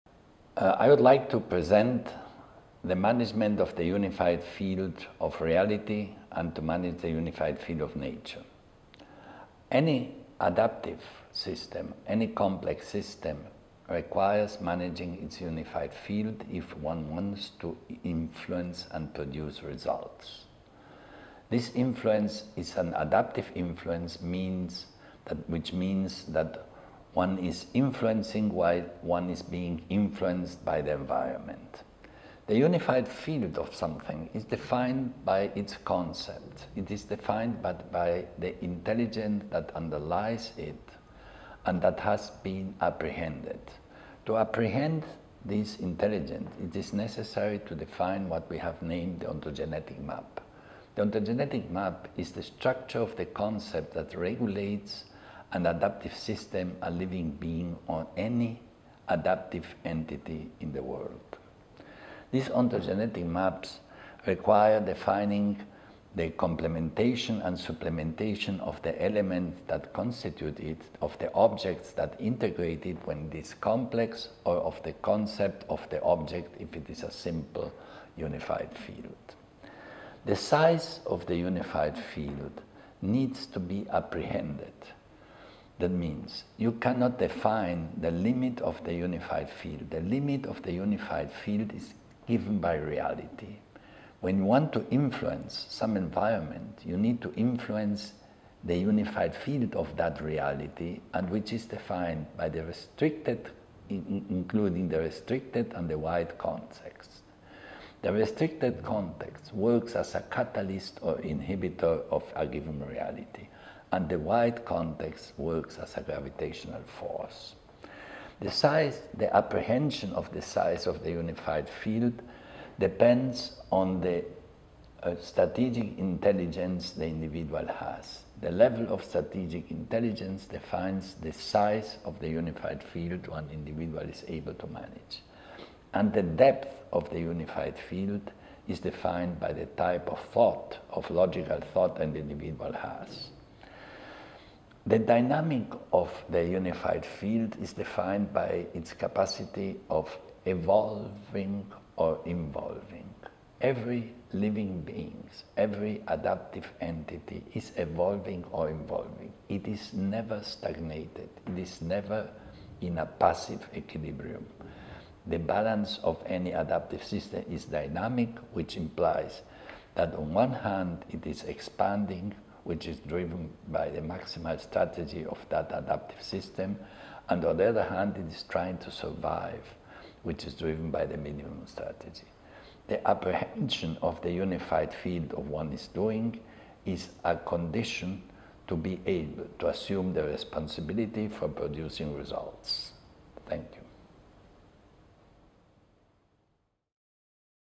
Unicist Unified Field Theory
By Peter Belohlavek. The Unicist Theory allows dealing with complex adaptive systems considered as a unified field to ensure the generation of value. It defines that the functionality of every entity of the real world is based on a purpose, an active function and an energy conservation function.